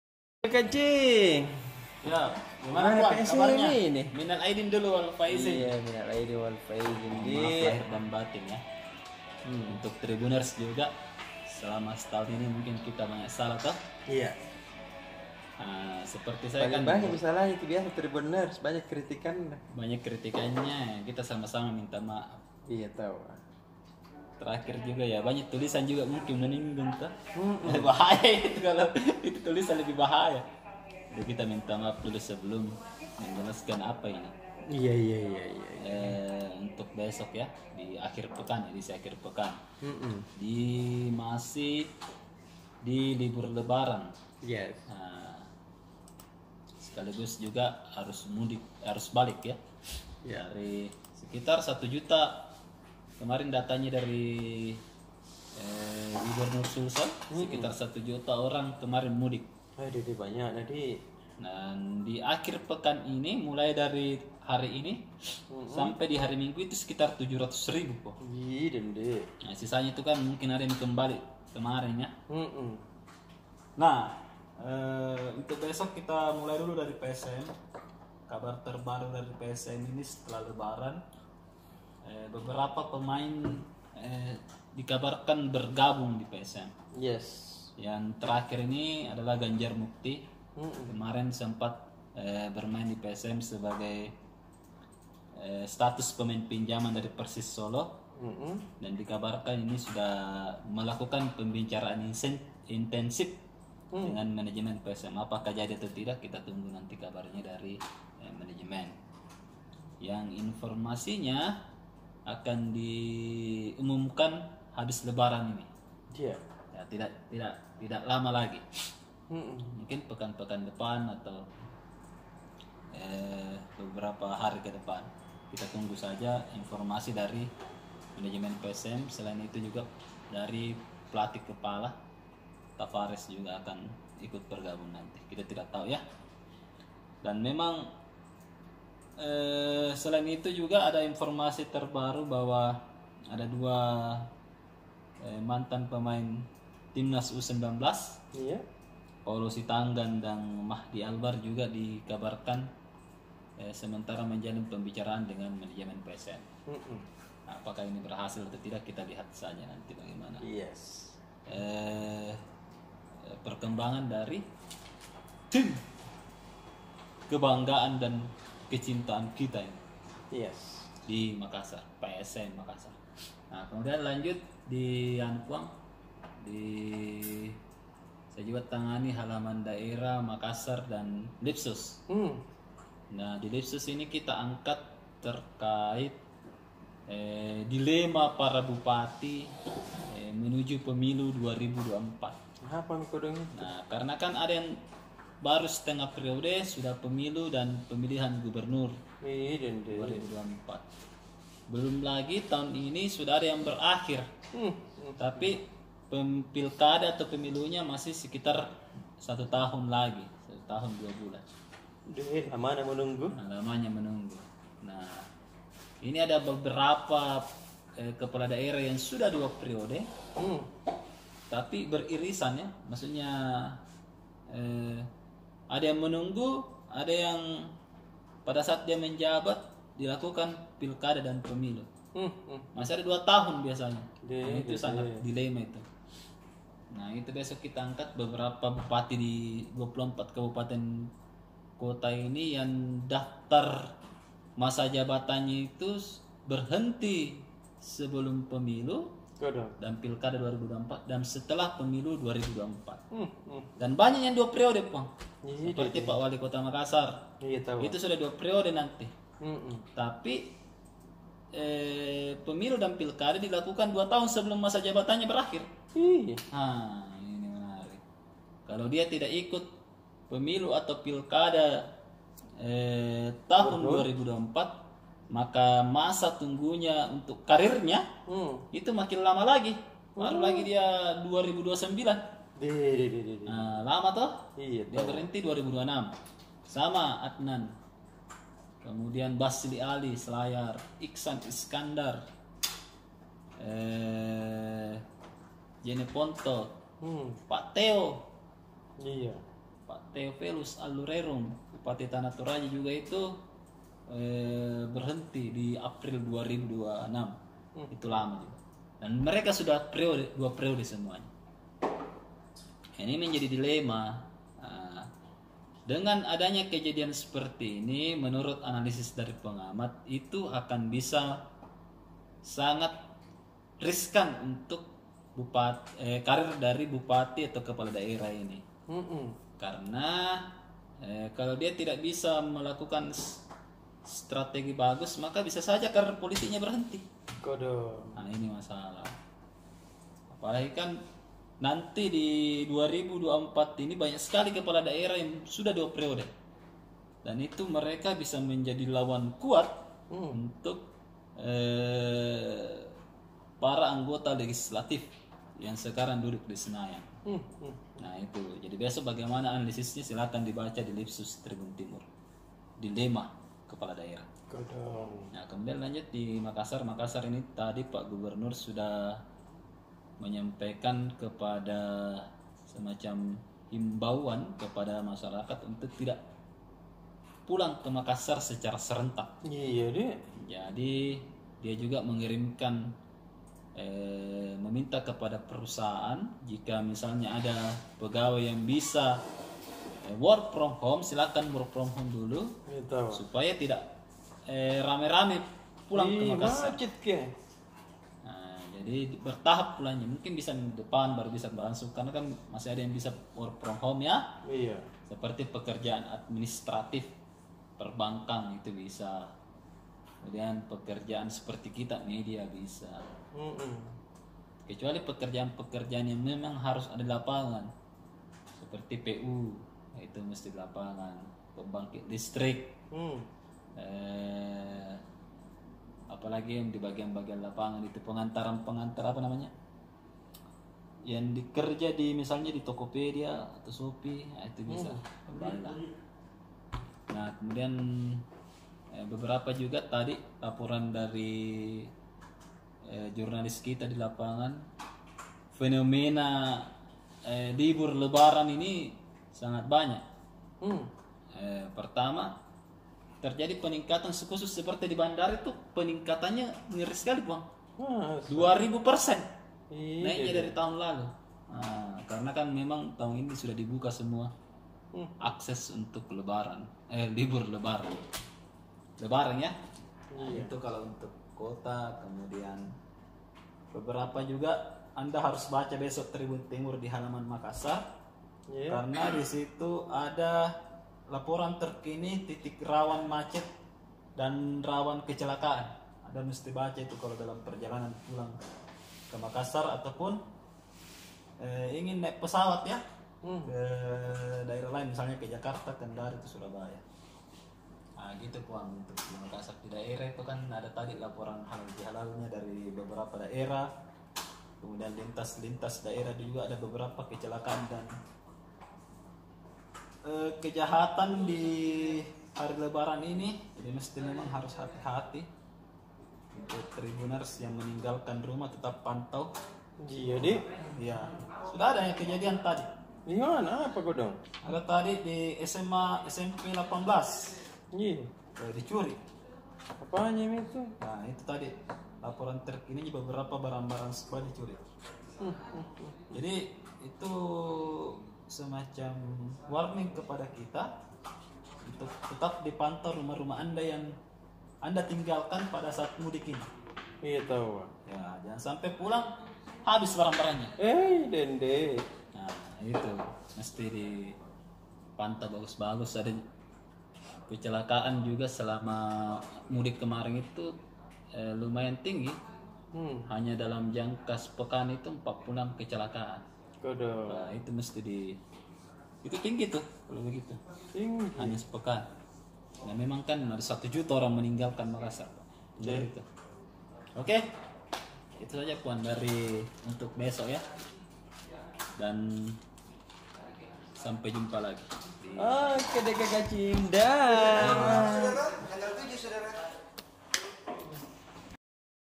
Ganjar Mukti Dekat ke PSM
Buru 2 Pemain Timnas U-19

PELAN tapi pasti beberapa pemain baru PSM sudah beredar. Bahkan, informasi Tribun, mantan pemain pinjaman gabung ke PSM.
Meski manajemen belum secara resmi memperkenalkan ke publik dan suporter.
Teranyar, bek tengah Persis Solo, Ganjar Mukti kini dikabarkan sudah menjalin kerjasama kontrak dengan Laskar Pinisi.
Selain itu, dua pemain mantan Tim Nasional U-19, gelandang, Paulo Sitanggang dan bek sayap kanan, Mahdi Albaar juga dikabarkan ingin direkrut PSM.
Ganjar memperkuat PSM pada putaran kedua Liga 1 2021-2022.
Namun, kala itu dia berstatus sebagai pemain pinjaman.
Sekarang,  pemain 27 tahun itu telah dilepas oleh Persis Solo bersama 24 punggawa lainnya.

(TRIBUN-TIMUR.COM)